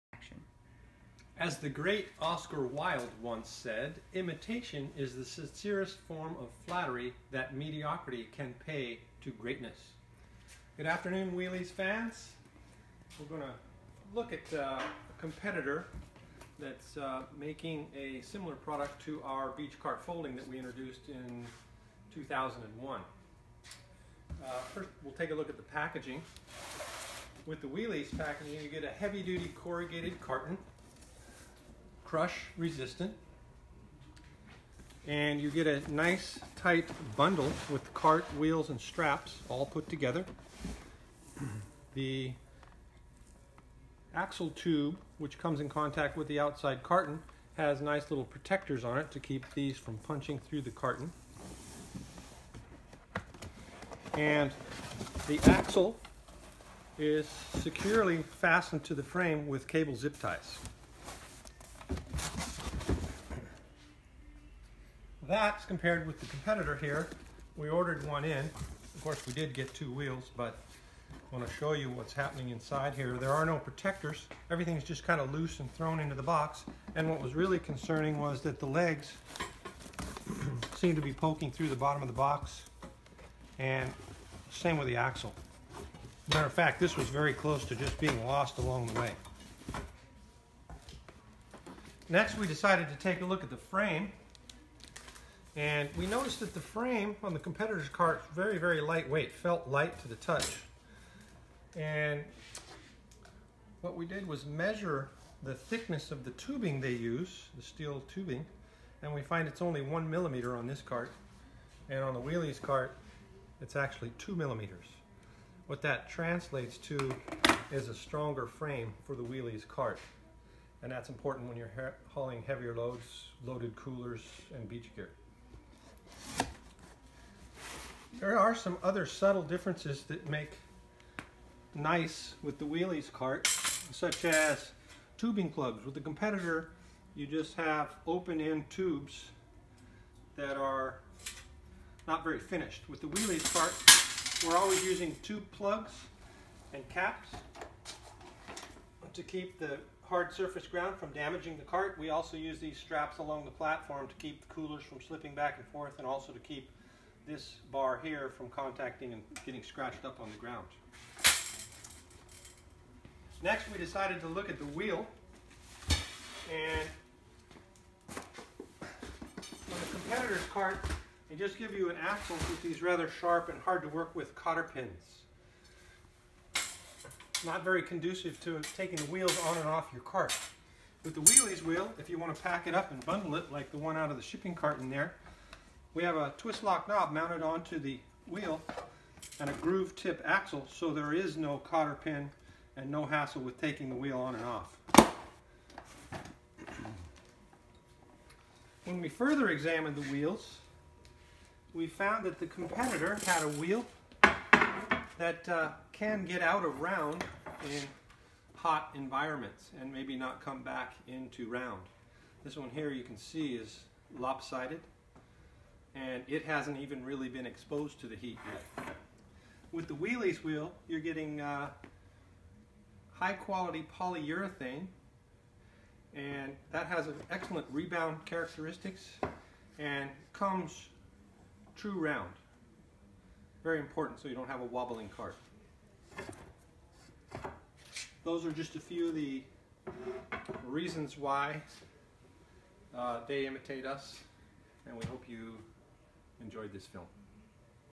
Wheeleez™ Beach Cart Folding vs the competition
This video provides an honest and complete comparison of the  Wheeleez™ Beach Cart vs the others available.